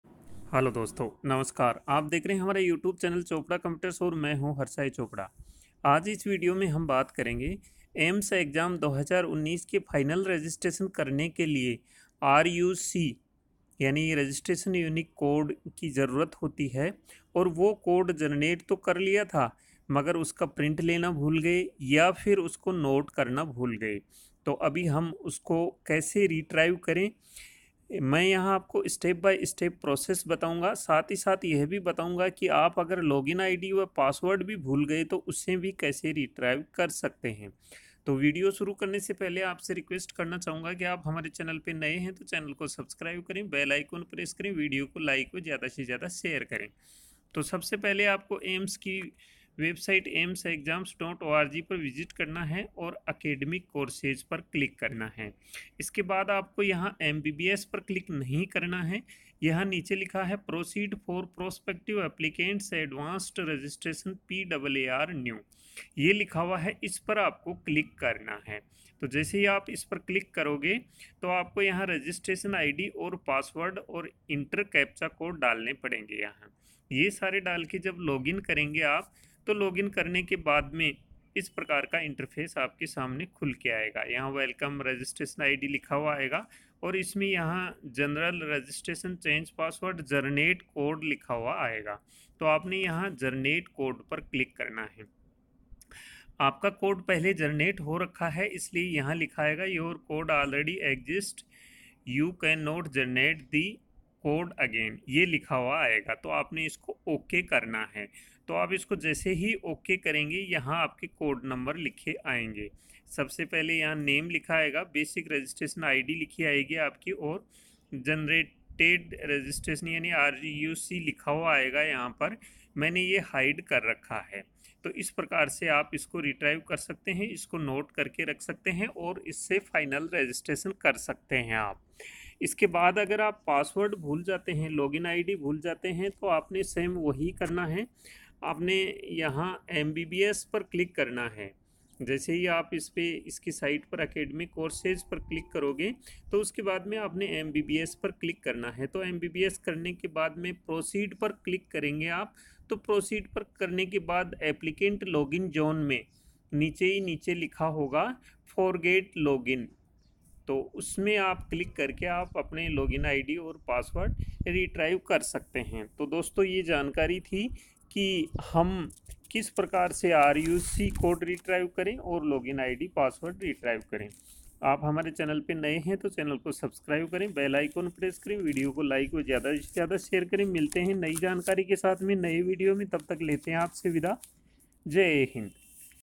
AIIMS MBBS exam 2019 !! How to generate code again
Hello Dosto , you are watching our YouTube channel "Chopra Computers" and I'm Harsahay Chopra.

In this video I telling about AIIMS MBBS exam 2019 !! How to generate code again.

Our Top most previous vedio links:-